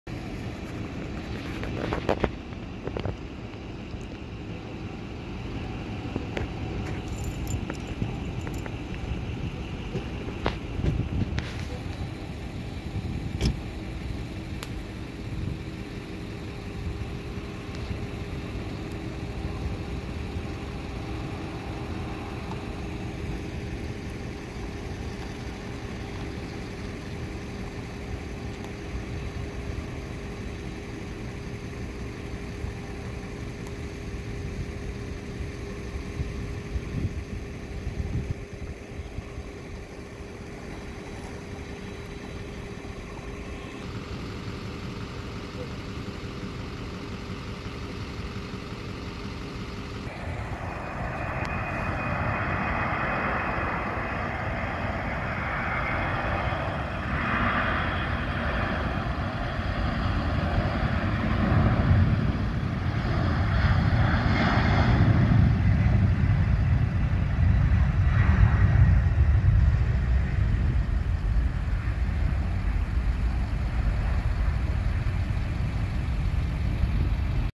944 Global SuperTanker. Take off. McClellan AFB
09/05/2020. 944 Global SuperTanker Take off at the McClellan AFB.  Retardant and fuel refill.